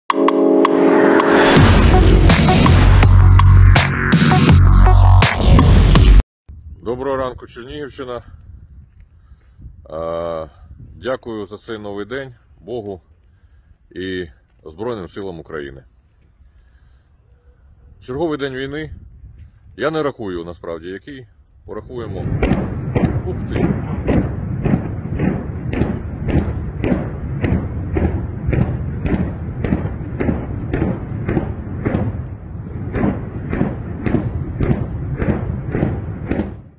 Все в ШОКЕ! Во время записи этого видеообращения случилось то, чего никто не ожидал...
Глава Черниговской ОГА Вячеслав Чаус запостил видеообращение, которое он записывал 21 марта, но что-то пошло не так.